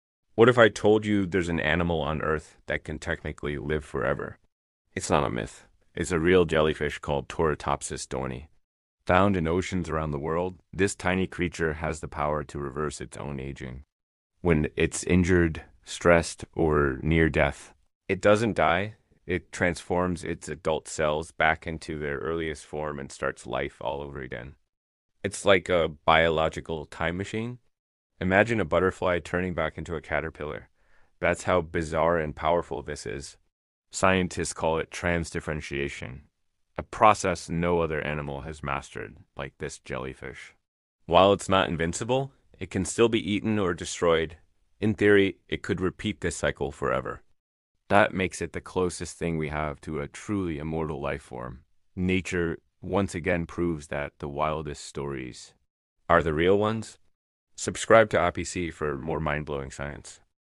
The Jellyfish That Can Live Forever 😱 | Immortal Animal Explained
It sounds like a sci-fi movie, but it’s real. Turritopsis dohrnii, also known as the immortal jellyfish, has a secret power no other animal on Earth has — it can reverse aging and restart its life cycle from scratch. Through a process called transdifferentiation, it transforms its mature cells back into a youthful state and lives again. While it’s still vulnerable to predators, technically, this jellyfish could live forever.